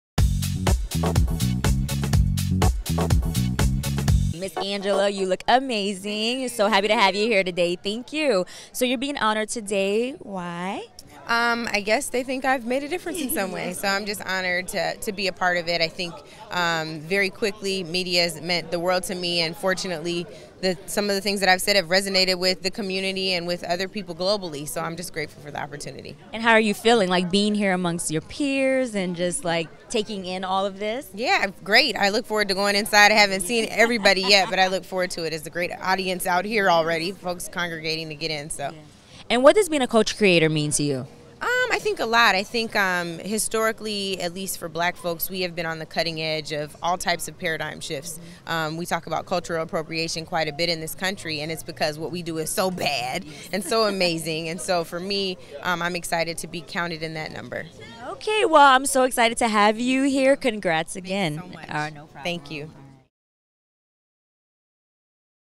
Angela Rye at the Culture Creators Brunch | VIBE
Angela Rye attends the Second Annual Culture Creators Brunch in LA.